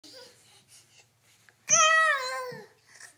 Sean Celebrates Olympics Hockey Going Into Overtime
Posted via email from aka @acarvin